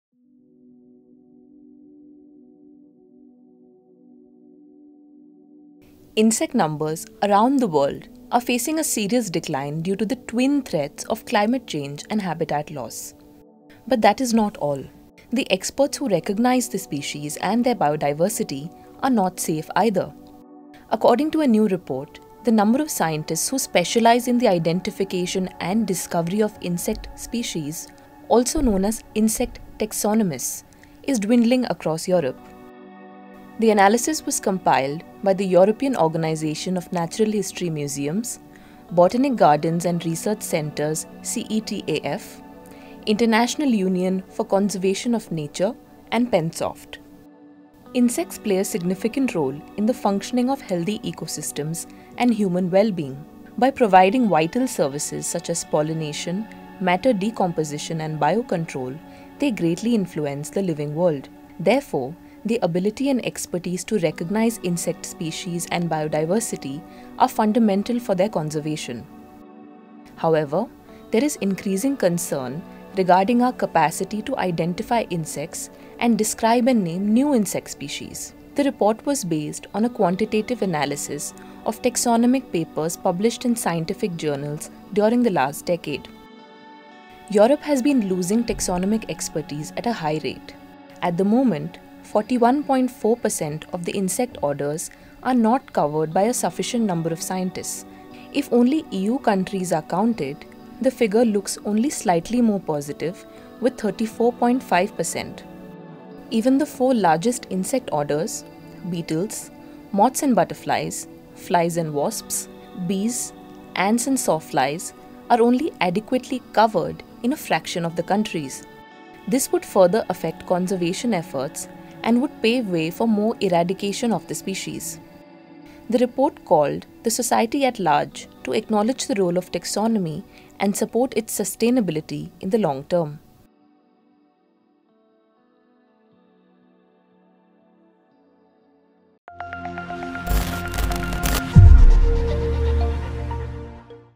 Insect taxonomy expertise in Europe may be at risk, says new study
Insect numbers around the world are facing a serious decline due to the twin threats of climate change and habitat loss. But now it's come to light that the experts who recognise the species and their biodiversity are not safe either.
According to a new report, the number of scientists who specialise in the identification and discovery of insect species — also known as insect taxonomists — is dwindling across Europe. The analysis was compiled by the European organisation of Natural History Museums, Botanic Gardens and Research Centers CETAF, International Union for Conservation of Nature (IUCN) and Pensoft*.
*The independent academic publishing company
Insects play a significant role in the functioning of healthy ecosystems and human well-being. By providing vital services such as pollination, matter decomposition, and bio-control, they greatly influence the living world. Therefore, the ability and expertise to recognise insect species and biodiversity are fundamental for their conservation. However, there is increasing concern regarding our capacity to identify insects and describe and name new insect species. The report was based on a quantitative analysis of taxonomic papers published in scientific journals during the last decade, as well as an online questionnaire.
Europe has been losing taxonomic expertise at a high rate.  At the moment, 41.4% of the insect orders are not covered by a sufficient number of scientists, the report found. If only EU countries are counted, the figure looks only slightly more positive with 34.5%.
Even the four largest insect orders: Beetles (Coleoptera), moths and butterflies (Lepidoptera), flies (Diptera) and wasps, bees, ants and sawflies (Hymenoptera) are only adequately ‘covered’ in a fraction of the countries.
This would further affect conservation efforts and would pave way for more eradication of the species. The report called the society at large to acknowledge the role of taxonomy and support its sustainability in the long term.